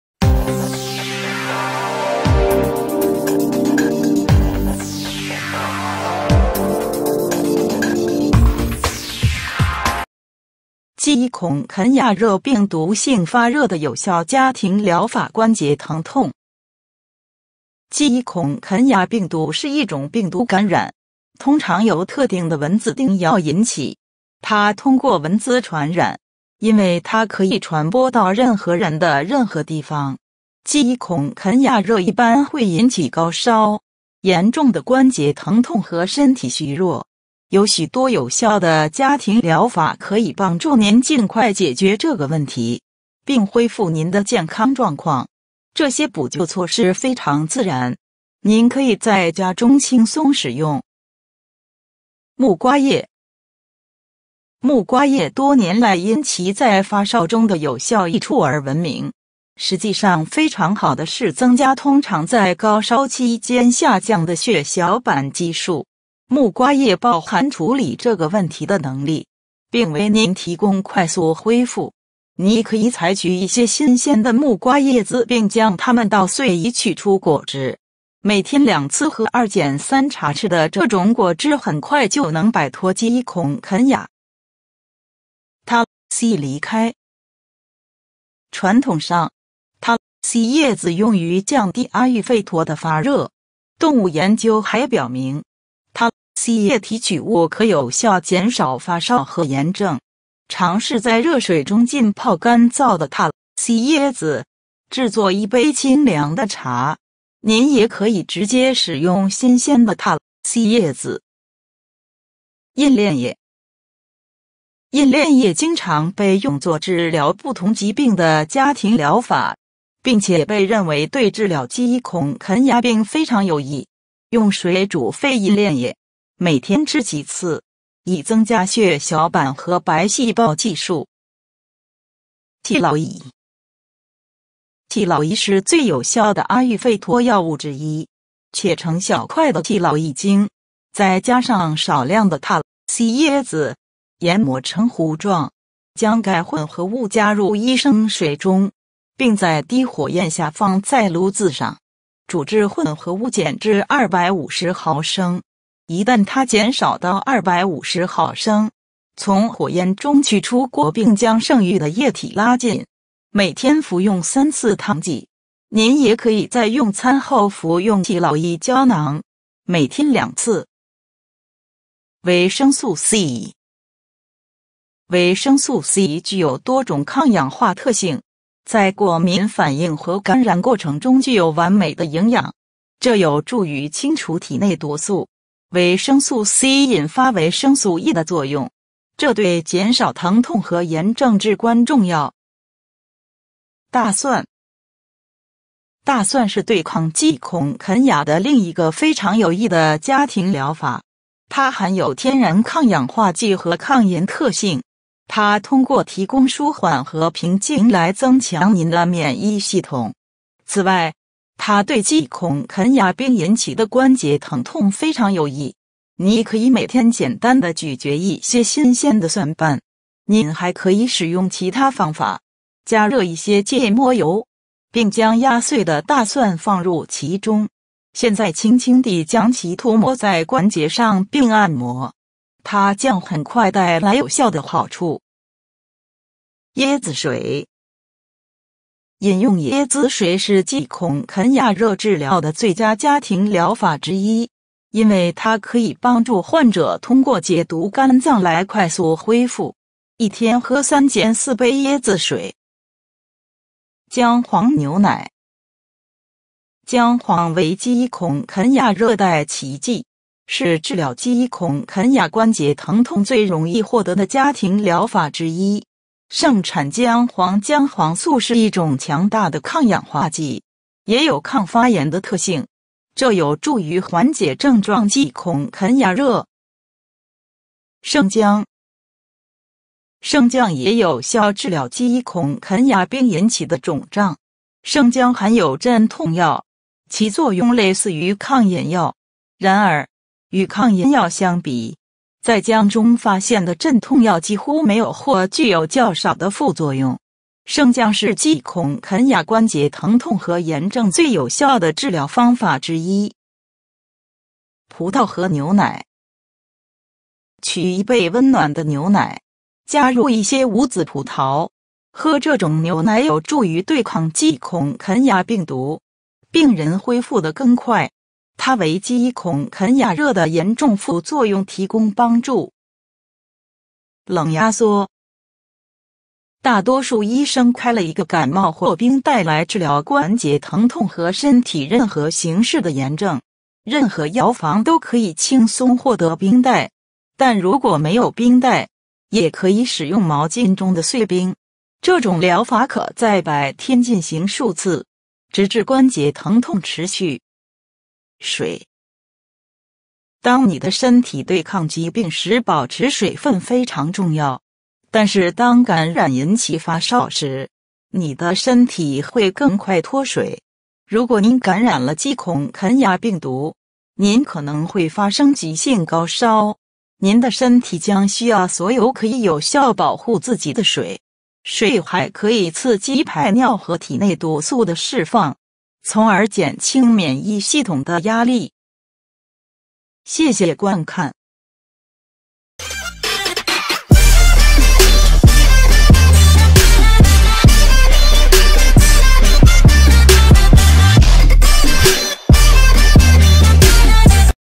基孔肯雅热病毒性发热的有效家庭疗法||关节疼痛
基孔肯雅病毒是一种病毒感染，通常由特定的蚊子叮咬引起。它通过蚊子传染，因为它可以传播到任何人的任何地方。基孔肯雅热一般会引起高烧，严重的关节疼痛和身体虚弱。有许多有效的家庭疗法可以帮助您尽快解决这个问题，并恢复您的健康状况。这些补救措施非常自然，您可以在家中轻松使用。